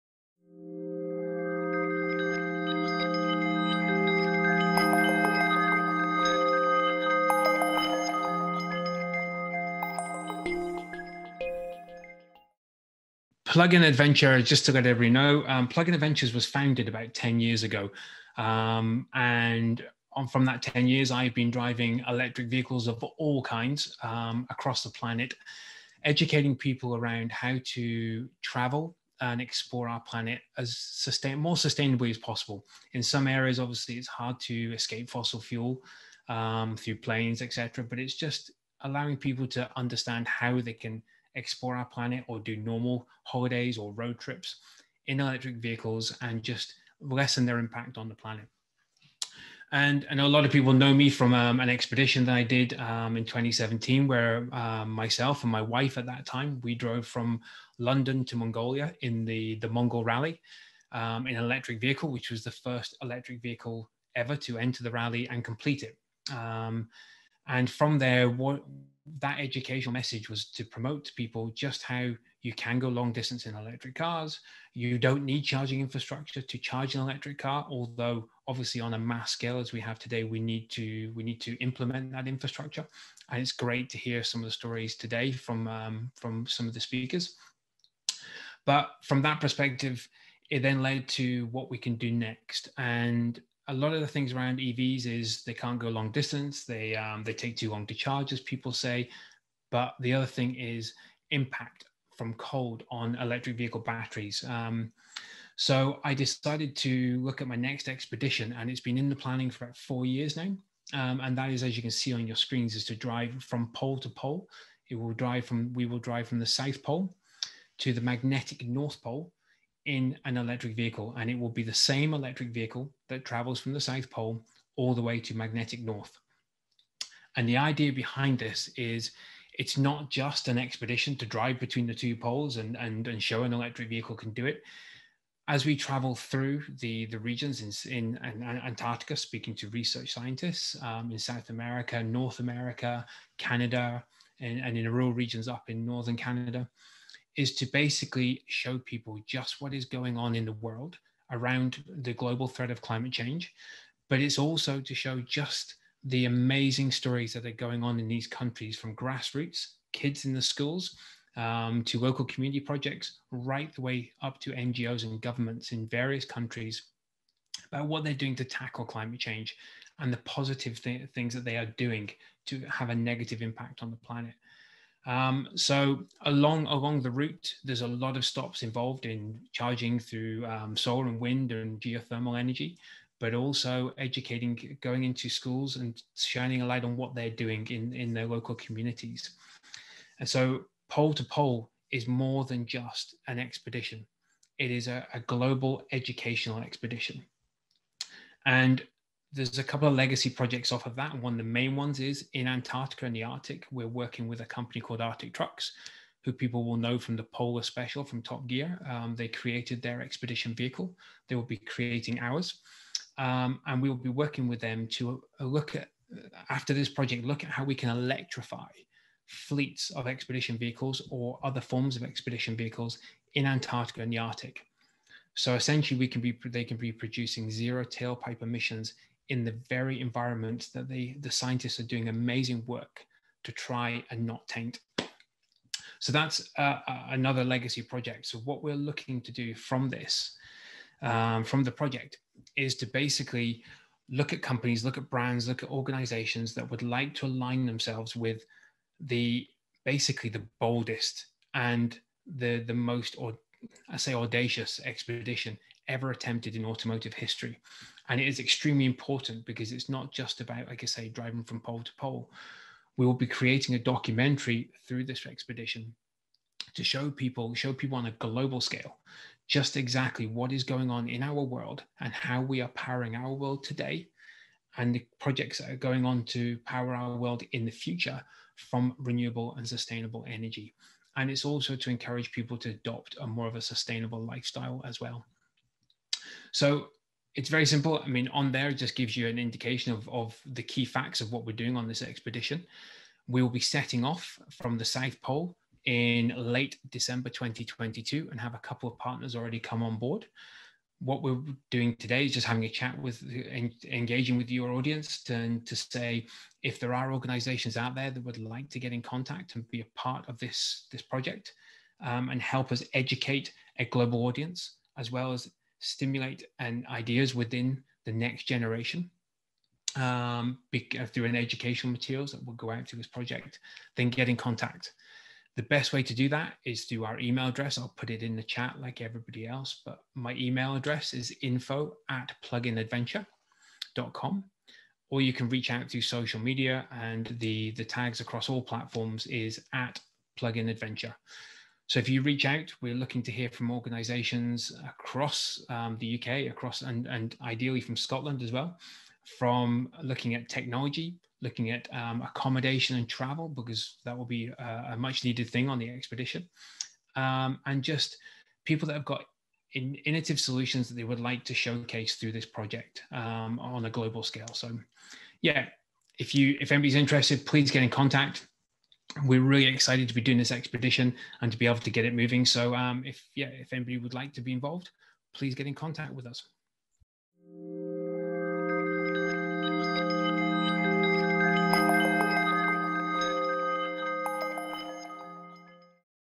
Plug in Adventures Opportunities
Event 2: Opportunities in Electric Mobility

Chris Ramsey from Plug in Adventures discusses the opportunity to get involved in his Pole to Pole adventure, raising awareness of climate change and supporting sustainable action.